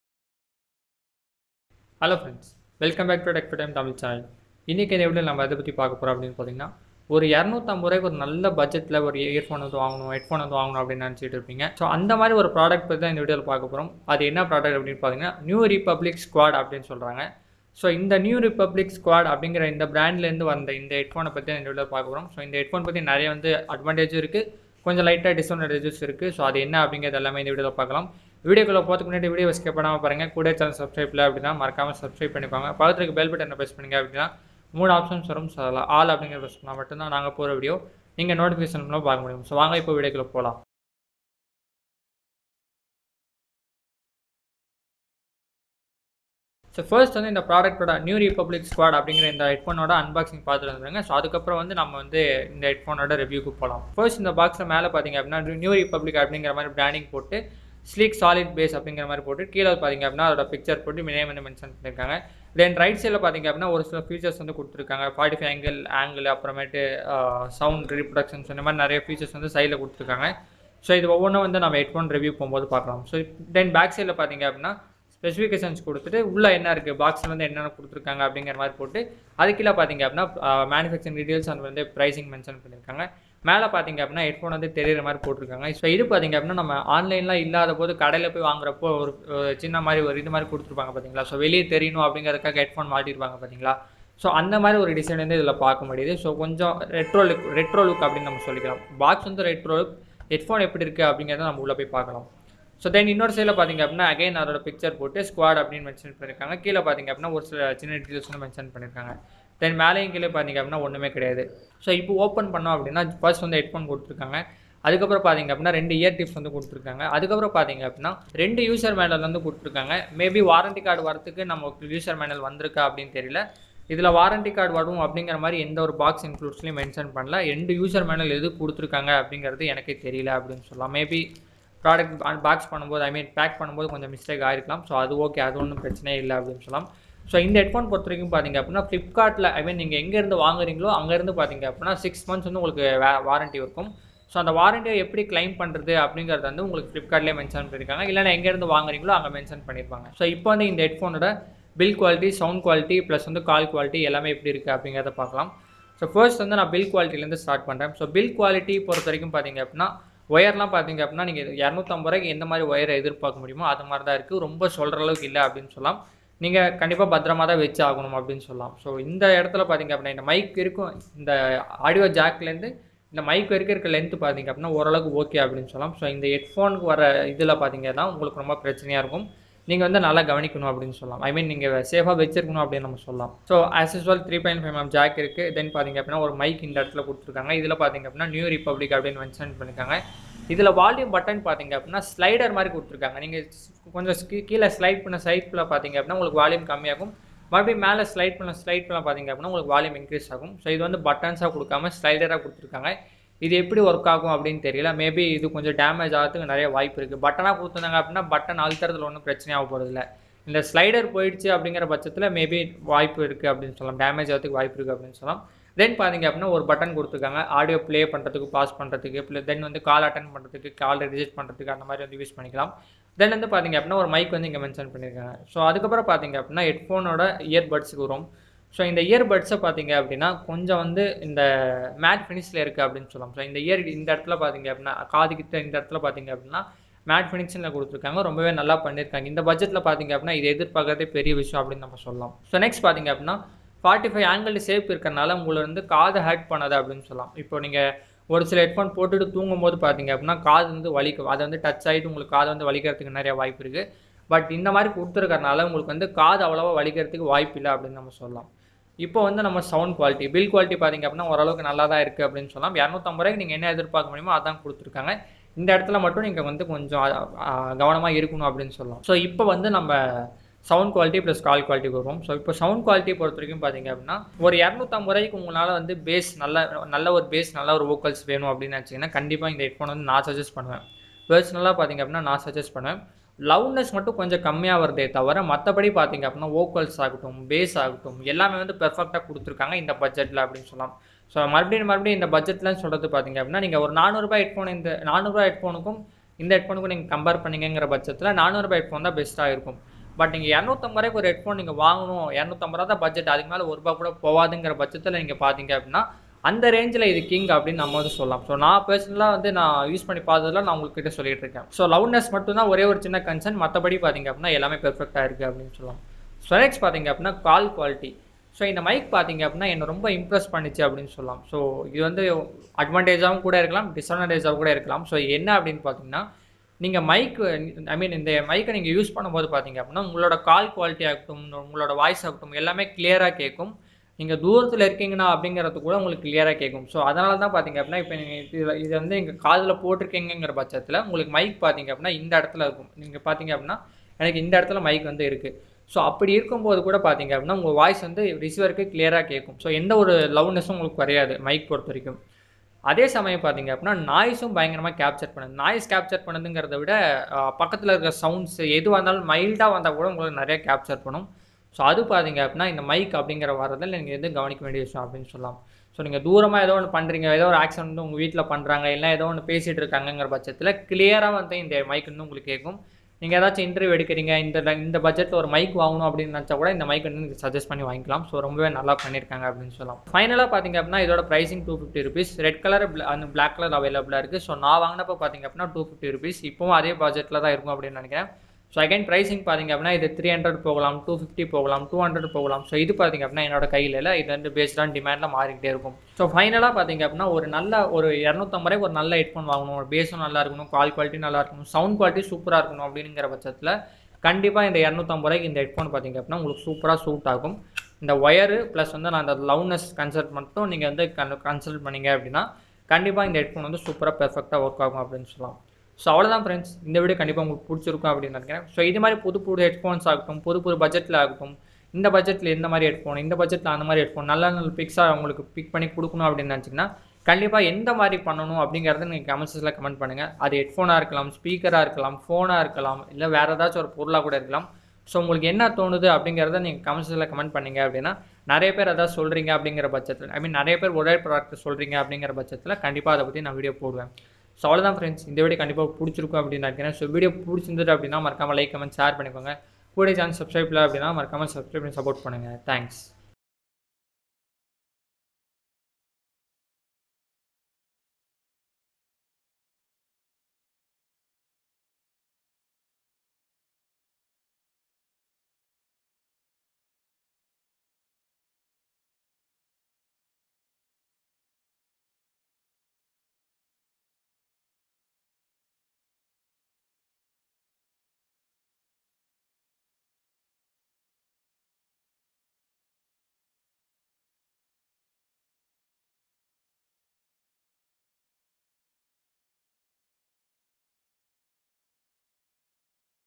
Nu Republic Squad Headphone Unboxing & Full Review in Tamil | Best Under 300 ₹🔥
Hi friends,
In this video we will unbox the nu republic squad headphone and review it in tamil

To get more updates about tech get in touch with twitter & instagram

Regards,
Tech for Time Tamil